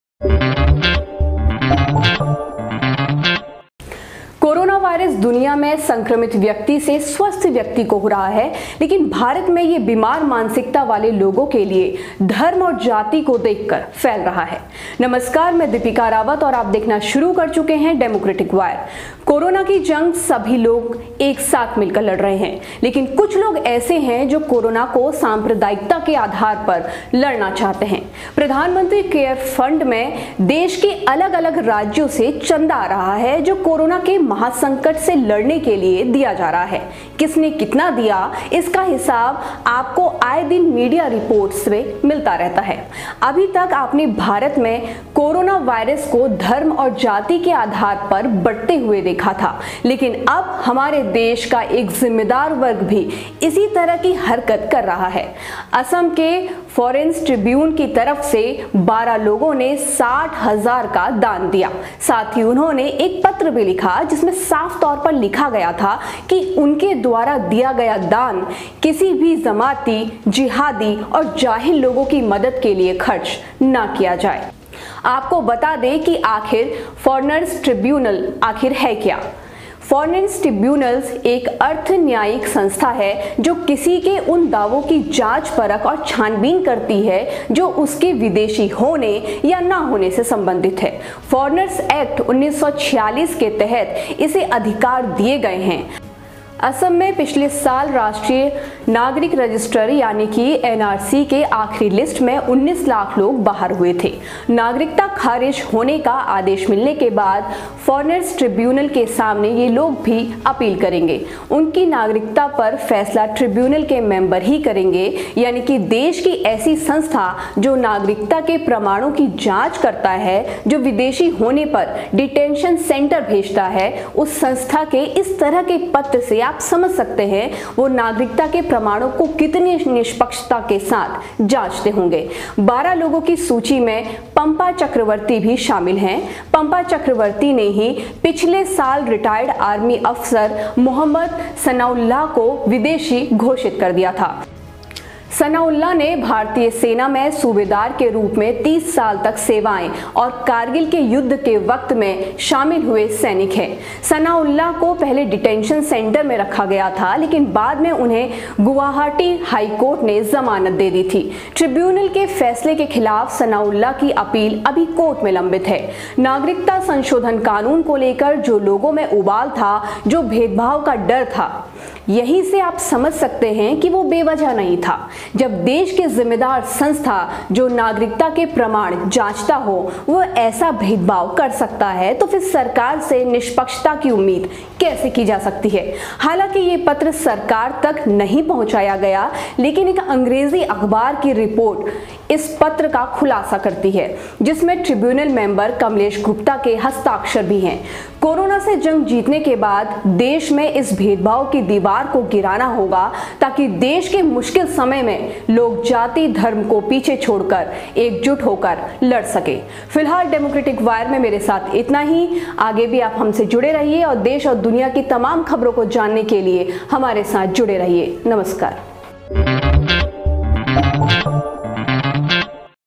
सनाउल्लाह को विदेशी घोषित करने वाले अफसर ने कोरोना राहत कोष में दिया दान, कहा-"जमातियों पर खर्च न हो
असम में देश की सेना के जांबाज अफसर रहे सनाउल्लाह को विदेशी घोषित करने वाले अफसर ने कोरोना राहत कोष में दिया दान, और कहा- " ये पैसा जमातियों पर बिल्कुल खर्च न हो"